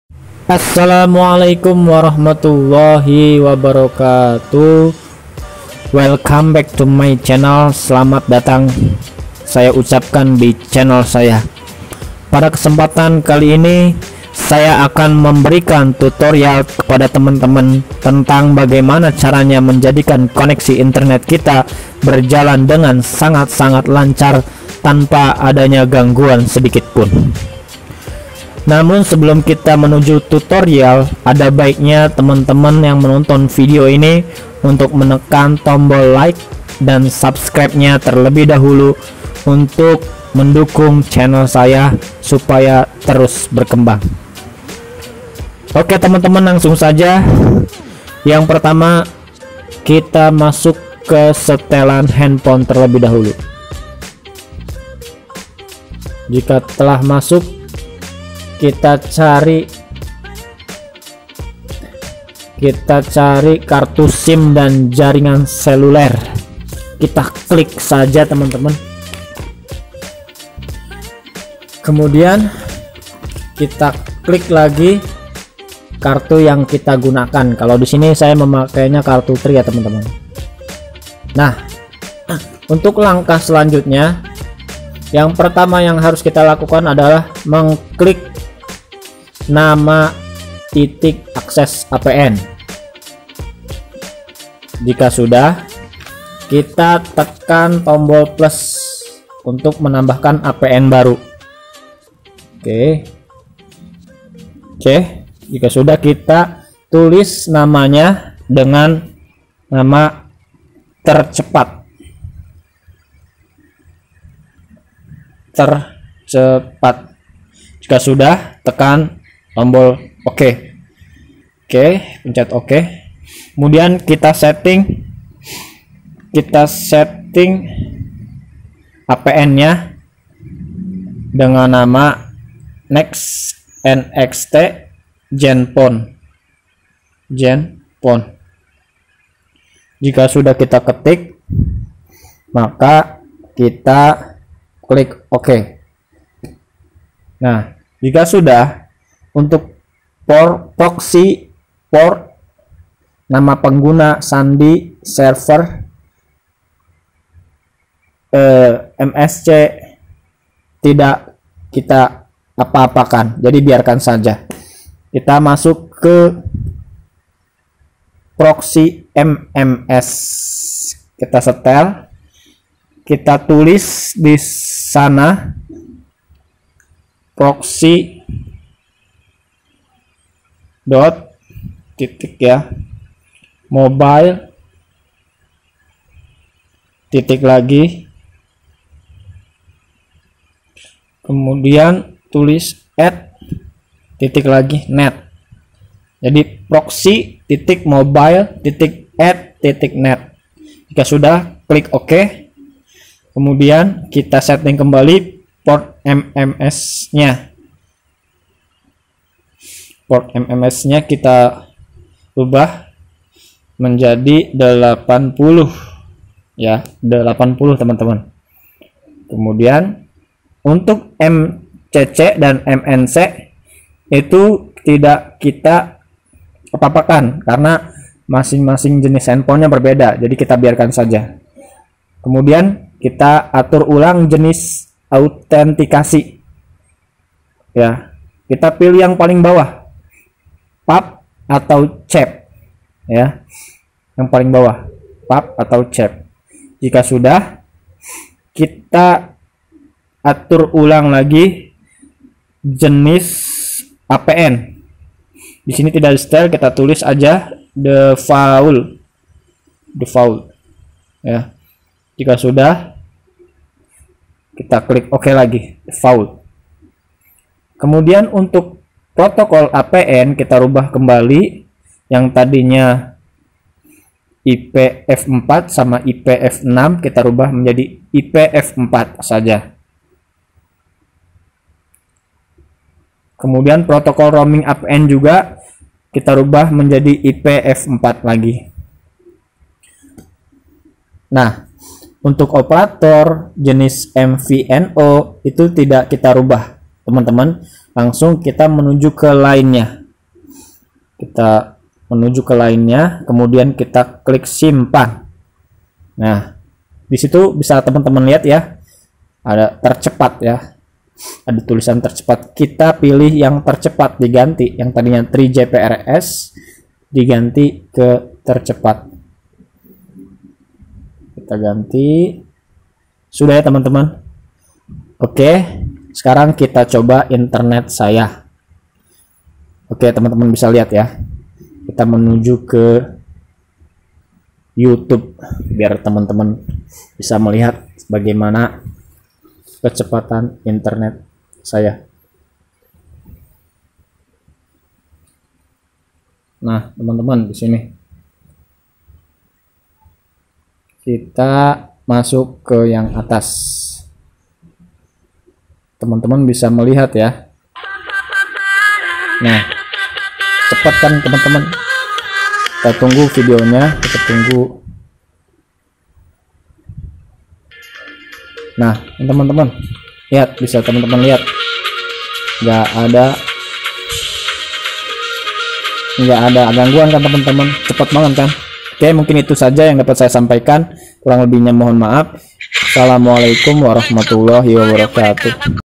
Cara mensetting koneksi Internet 4G Ke 5G Tanpa Aplikasi 2021
#5 G
tutorial menjadikan koneksi Internet 4G LTE menjadi Rasa 5G Tanpa Aplikas